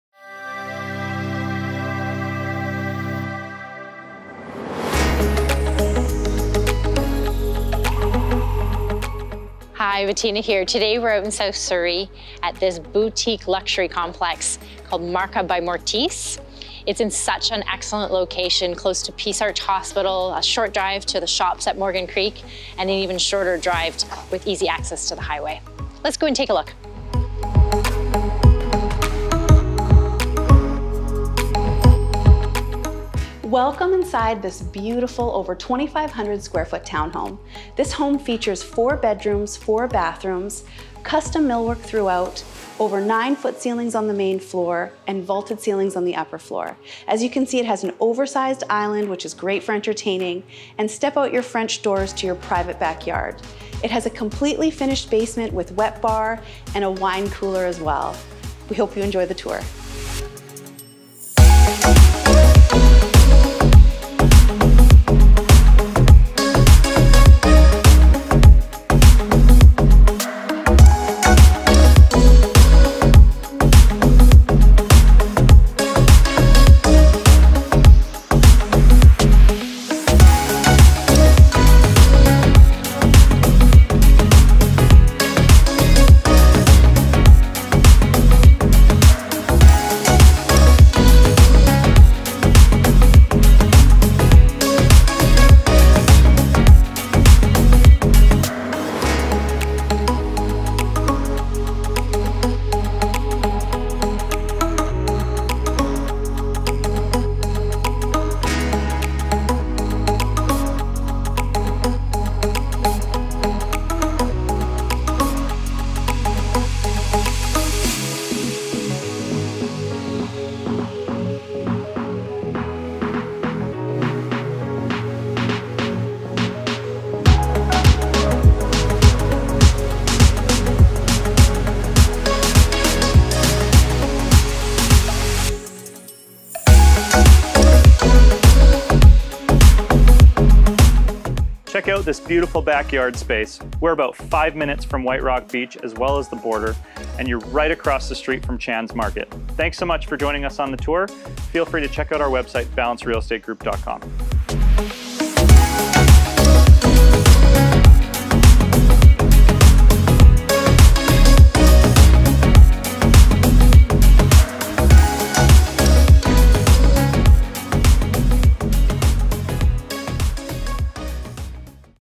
#6 1454 162B St, Surrey - Real Estate Video Tour - Balance Real Estate Group
View to see PRICE and more PHOTOS.

Executive Townhouse in boutique complex - Marca by Mortise. This close to 2500sf home features laminate floors on the main. Great room style floorplan with exposed beams in the vaulted ceilings. Stunning entertainers style kitchen. Large eat up bar island with quartz counters and extra custom cabinet added on to the end. SS appliances including a gas range. Plenty of cabinets in 2 tones with black hardware. 3 generous sized bedrooms up. Spacious primary bedroom with vaulted ceilings, walk in closet and barn door slider into expansive ensuite with dual sinks and double wide shower. Dual patio doors off primary bedroom lead out to east facing deck, perfect place to enjoy a morning coffee. Fully finished basement with 4th bedroom, full bathroom and media room with wine fridge counter and sink. Bonus Hot water on demand and AC for those hot summers. Excellent location with easy access to HWY 99 into Vancouver or close to the border. Only a few blocks from the beach and short drive to all your shopping needs.

Bedrooms: 4
Bathrooms: 4
SqFt: 2435